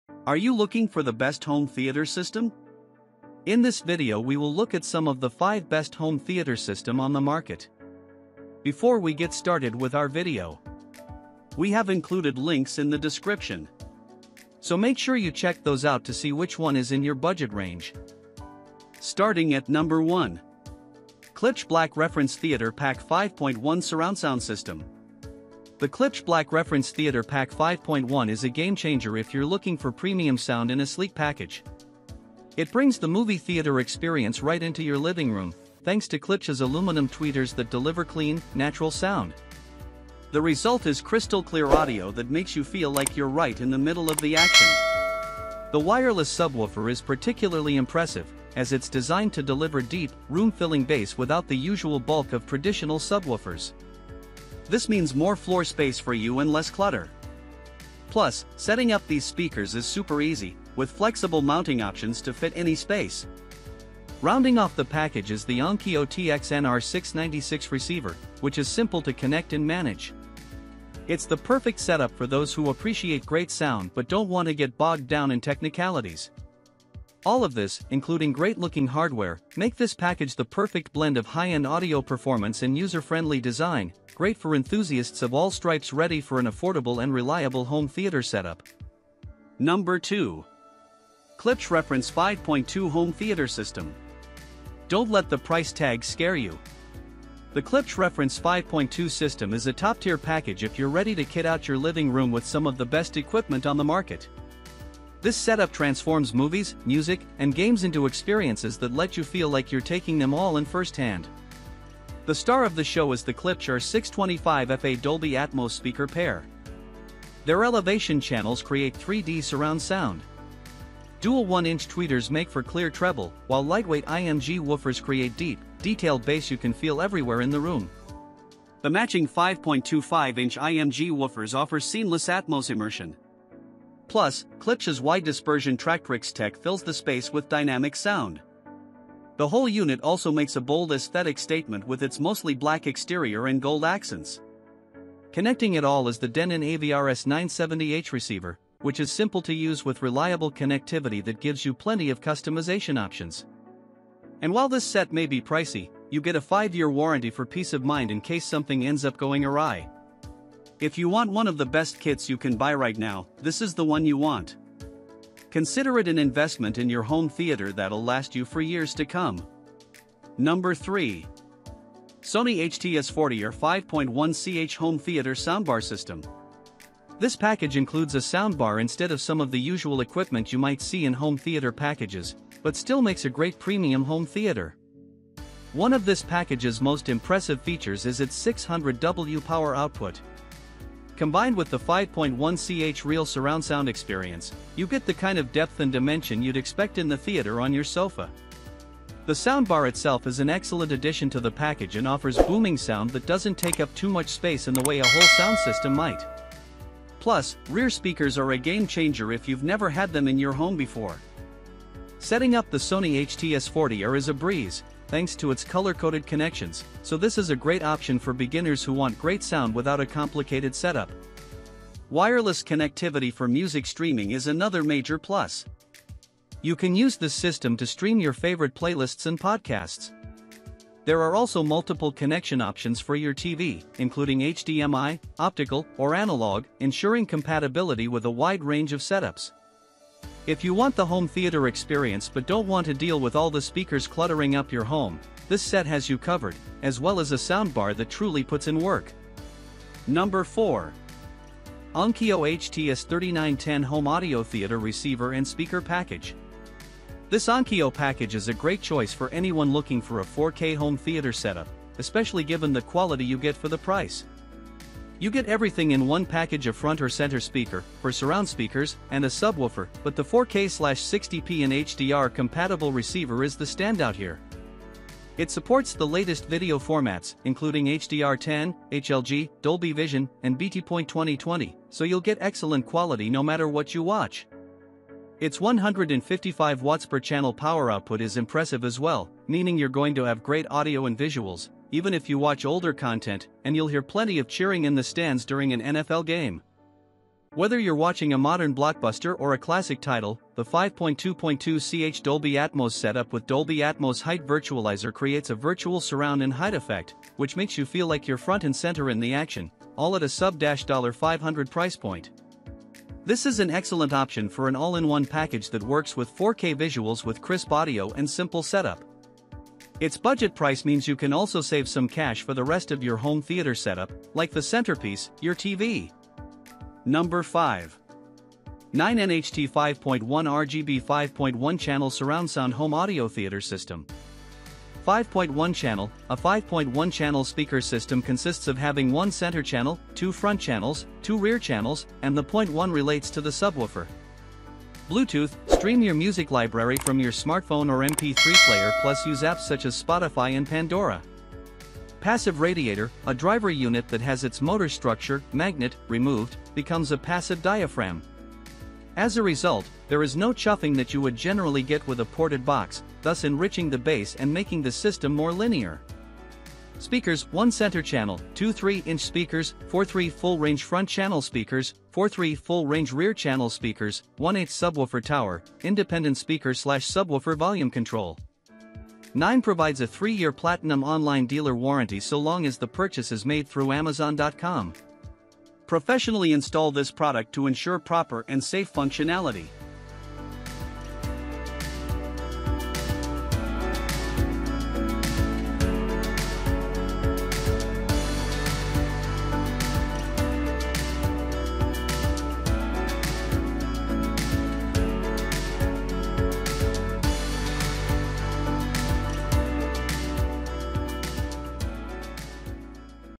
TOP 5: Best Home Theater System in 2024 👌
Hey guys in this video we are going to be checking out the Best Home Theater System. You Can Buy Right Now. We made This List Based On Our Personal Opinion and hours of research & we have listed them based on the type of features and Price. We have Included options for every type of user so whether you are looking for the best budget.

      owners.